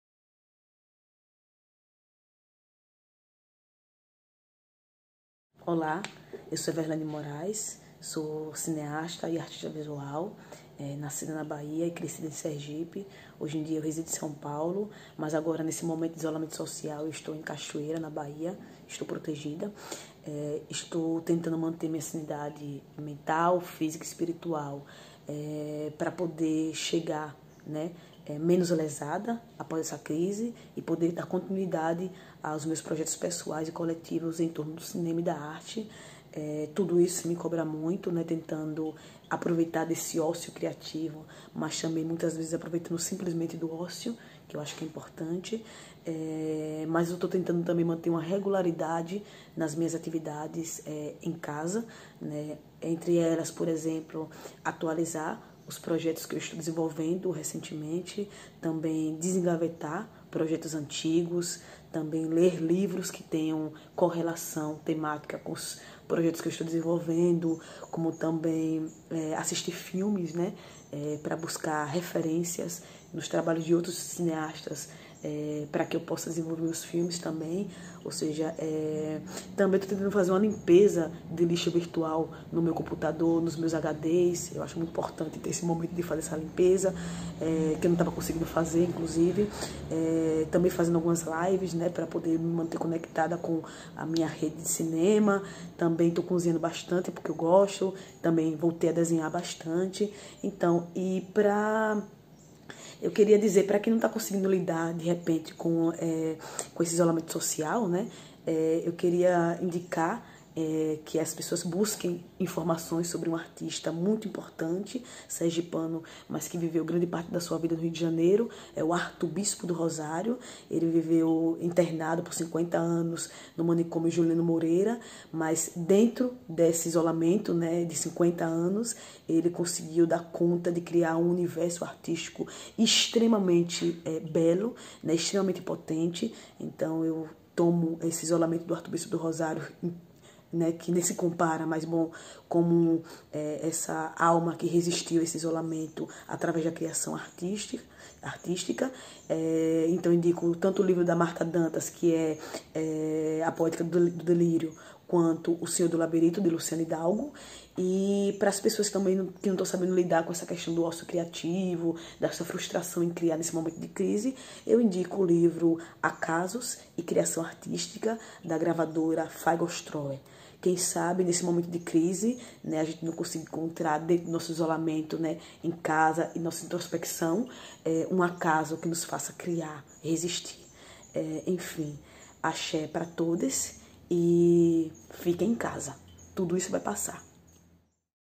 Insurgente Poética Everlane Moraes
Vamos de cinema hoje?

A convidada insurgente de hoje é a fundadora e co diretora do grupo criativo de autores A Irmandade Filmes, Everlane Moraes. Membra da A.P.A.N. (Associação dos Profissionais do Audiovisual Negro) e especializada em Direção de Documentário pela Escuela Internacional de Cine y TV (EICTV/Cuba). Graduanda em Artes Visuais - Licenciatura pela Universidade Federal De Sergipe (UFS/Brasil), Everlane nos presenteia com um relato cheio de otimismo e nos convida a entender o tão comentado ócio criativo nessa quarentena.

Sua assinatura cinematográfica apresenta uma estética híbrida, que dialoga com as artes visuais e o cinema em seus diferentes gêneros. Nesse espaço de experimentação fílmica, trabalha com os conceitos filosóficos ligados às questões socioculturais da diáspora negra em busca de retratar as identidades e subjetividades dos seus personagens.

Em 2019 a cineasta foi seleciona para o Encontro entre Diretores no 34ª Talents Guadalajara em parceria com o Berlinale Film Festival. Com trabalhos cinematográficos sendo exibidos em diversos festivais de cinema e artes no Brasil e no exterior, incluindo Sundance Film Festival, Toulouse Film Festival, BFI London Film Festival, Rotterdam Film Festival, True and False Film Fest, Contemporany Latin American Documentary Film Festival, Documenta Madrid e Mostra de Direitos Humanos da América Latina, a cineasta disponta como uma grande promessa do cinema nacional e uma brilhante representante do ativismo cultural negro.

𝐄𝐯𝐞𝐫𝐥𝐚𝐧𝐞 𝐜𝐢𝐭𝐚:

Livro ‘’Arthur Bispo do Rosário: a poética do delírio’’ de Marta Dantas pela Editora Unesp
Livro ‘’Arthur Bispo do Rosário: O senhor do labirinto’’ de Luciana Hidalgo pela Editora Rocco
Livro ‘’Acasos e Criação Artística’’ de Fayga Ostrower pela Editora da Unicamp